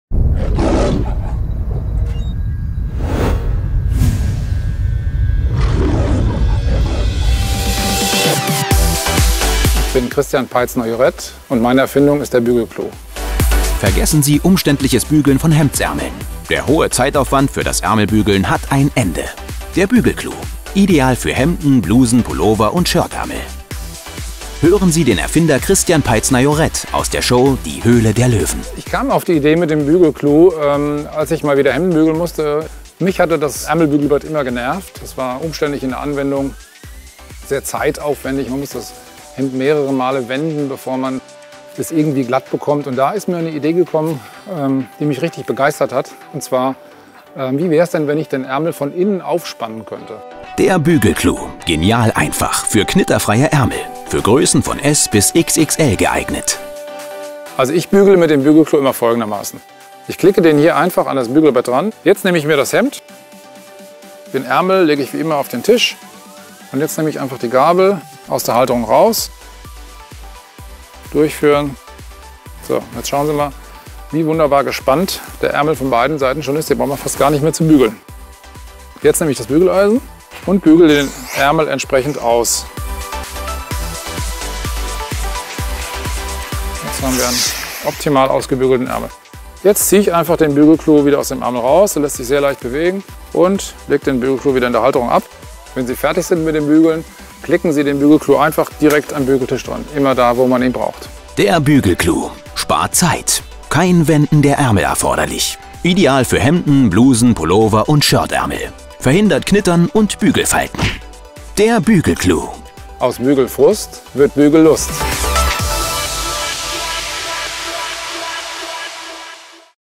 Bügelhilfe Bügel-Clou 600 mm Edelstahl für Ärmel (00022) - "Die Höhle der Löwen"
Genial einfach! Jetzt gelingt knitterfreies Ärmelbügeln in Sekundenschnelle! Der „BÜGEL-CLOU“ ist die unkomplizierte Bügelhilfe für klassische Hemden- und Blusenärmel. Geformt aus einem flexiblen Federstahldraht passt sich der „BÜGEL-CLOU“ Ihrem Ärmel an – ideal für alle Größen von S-XXL! Dank der Spreizspannung und der unterschiedlich langen Schenkel sitzt die Federgabel im Ärmel optimal und spannt ihn auf. So bügeln Sie in einem Arbeitsgang, ganz ohne Wenden des Stoffs: Das spart Zeit und schont die Fasern! Mit der magnetischen Halterung können Sie die „BÜGEL-CLOU“ Bügelhilfe einfach zum Ablegen und Verstauen am Bügelbrett befestigen. Genial einfach, einfach genial: Die „BÜGEL-CLOU“ Bügelhilfe ist der schnelle Weg zu knitterfreien Ärmeln!